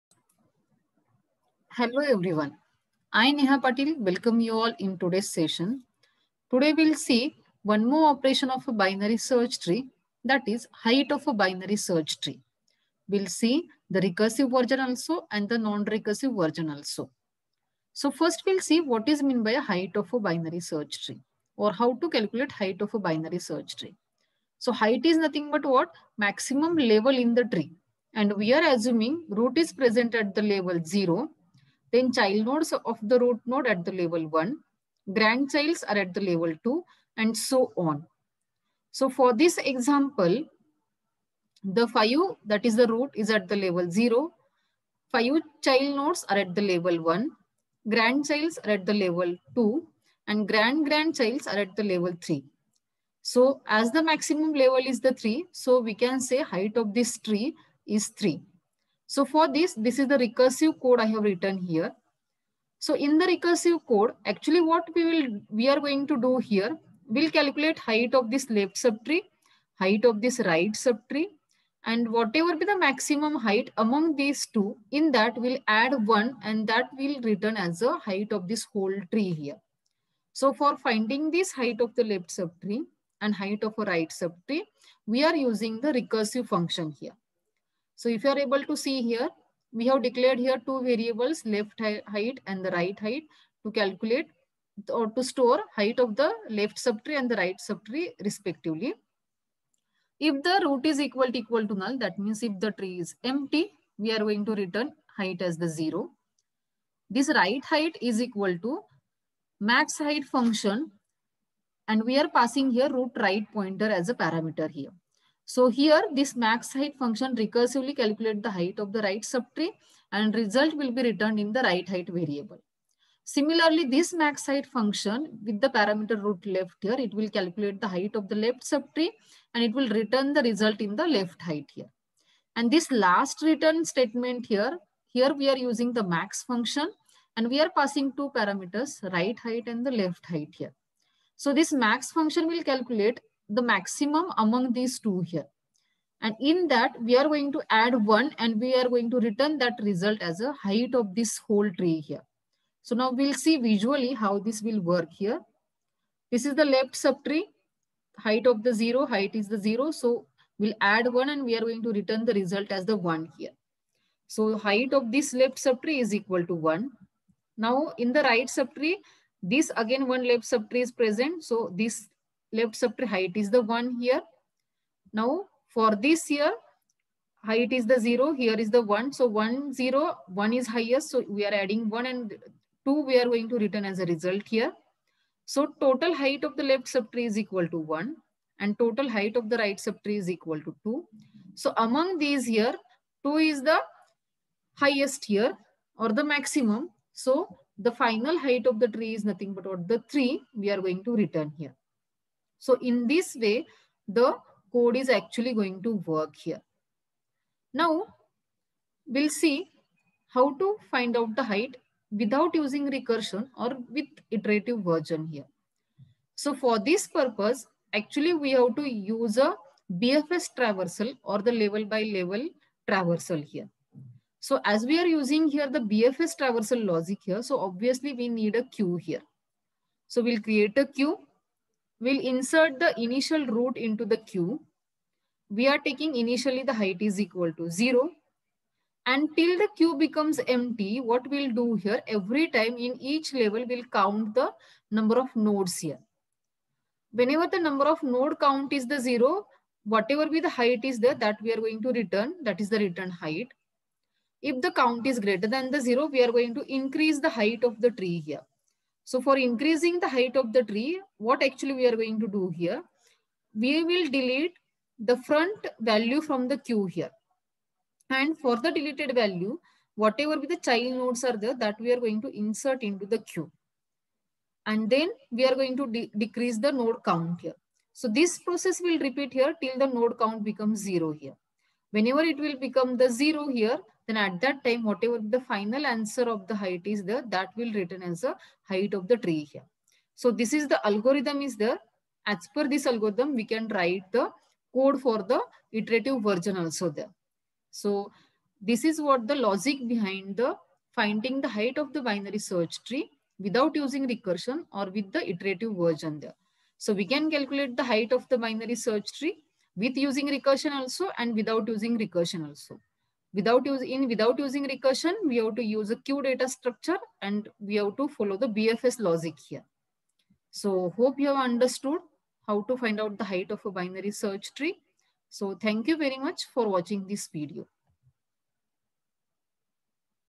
Finding Height of Binary Search Tree
Recursive and Non Recursive code to find height of binary search tree